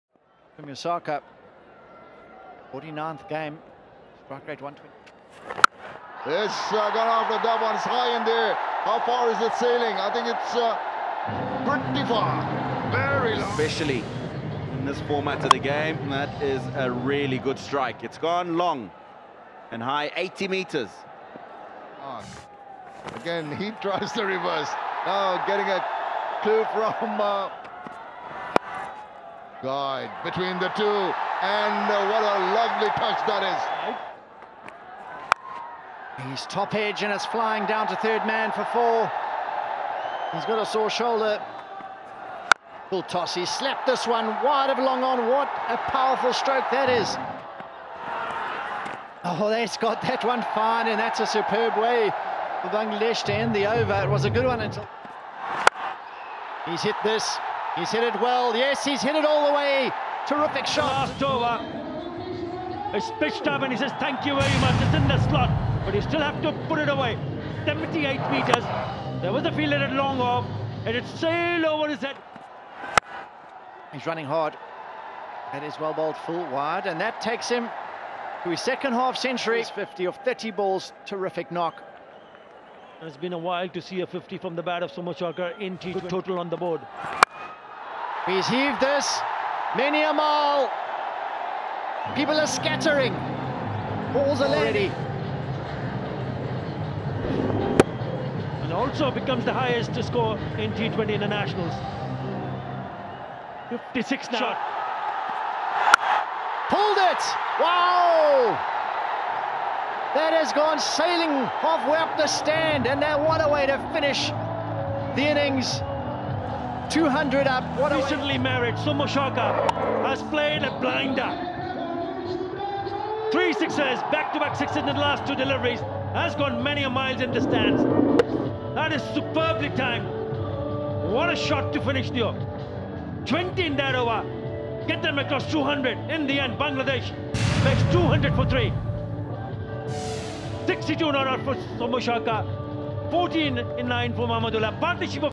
Soumya Sarkar's 62 Runs Against Zimbabwe | 1st T20I | Zimbabwe tour of Bangladesh 2020
Series : Zimbabwe tour of Bangladesh, 2020
Match Date : March 09, Monday 2020.
Venue: Shere Bangla National Stadium,Dhaka.